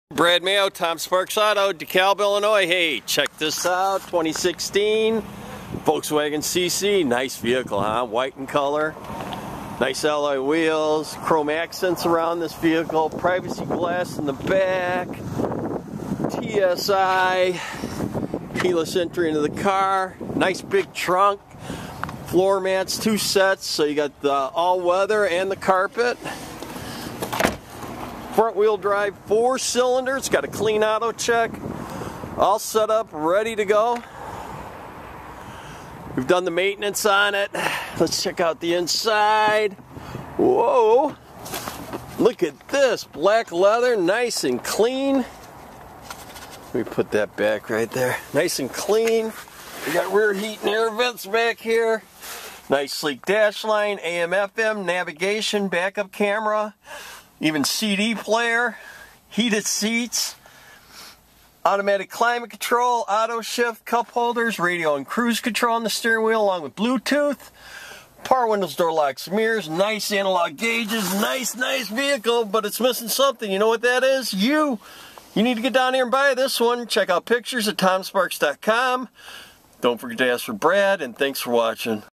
2016 Volkswagen CC DeKalb Il Near Sycamore Il
NAVIGATION SYSTEM, HEATED FRONT SEATS, TURBOCHARGED ENGINE,PREMIUM SYNTHETIC SEATS, DUAL POWER SEATS, POWER HEATED MIRRORS, KEYLESS START, KEYLESS ENTRY, CRUISE CONTROL, BLUETOOTH, SMART DEVICE INTEGRATION, ALUMINUM WHEELS, RAIN SENSING WIPERS, HID HEADLIGHTS, FOG LAMPS, BACK UP CAMERA, ALUMINUM WHEELS, SATELLITE RADIO READY, MULTI-ZONE CLIMATE CONTROL.